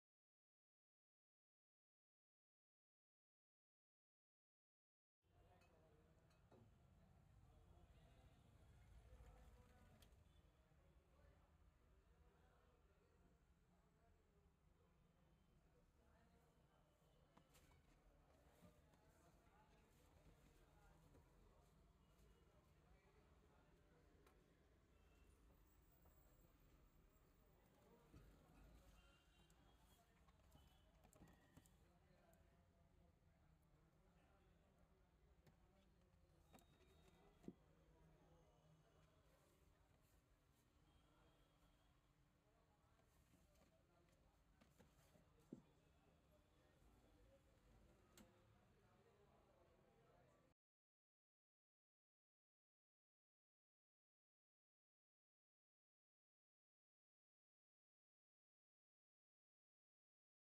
Do You know - How much alcohol and water is present in whisky bottles.
Have we ever thought why its written 42.8 % v/v in every alcohol bottle we buy. There is a general assumption that whisky bottle we are buying is that entire quantity as alcohol but we help you in removing such misconceptions.